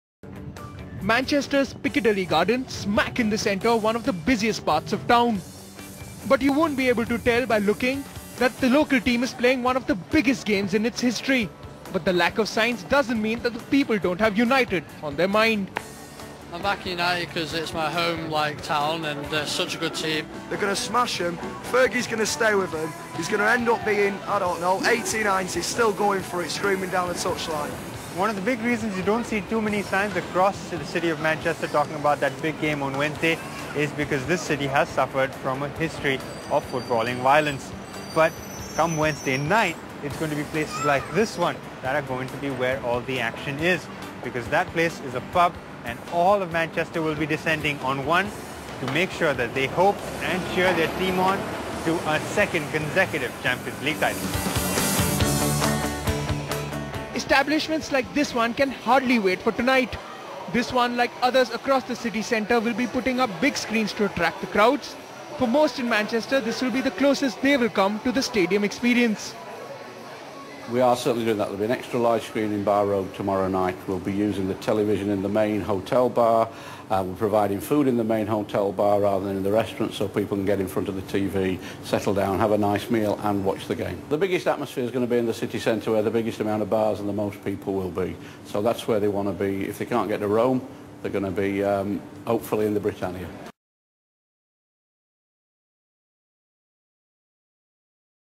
Man U fans all set for the League final
Manchester United fans get ready for the big clash - the Champions League final - against Barcelona on Wednesday.